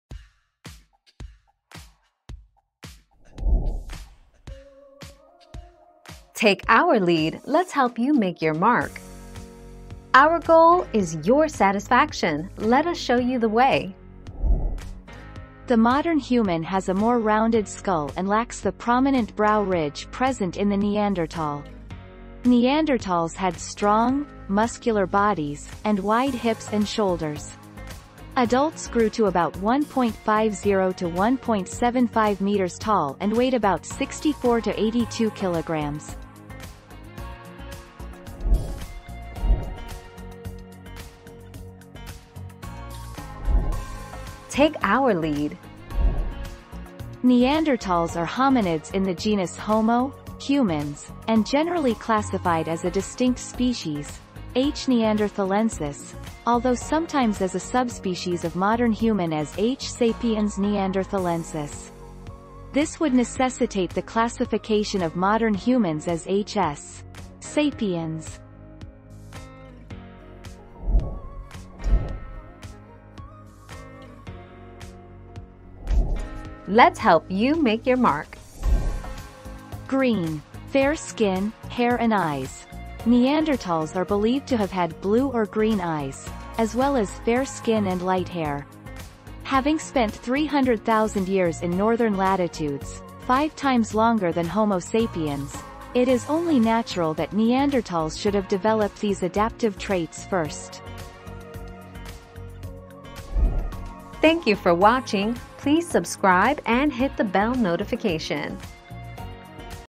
What is a Neanderthal vs human?
00:00 - What is a Neanderthal vs human?
00:40 - Is a Neanderthal a human?
01:12 - What color eyes did Neanderthals have?

Laura S. Harris (2021, April 10.) What is a Neanderthal vs human?
    AskAbout.video/articles/What-is-a-Neanderthal-vs-human-234269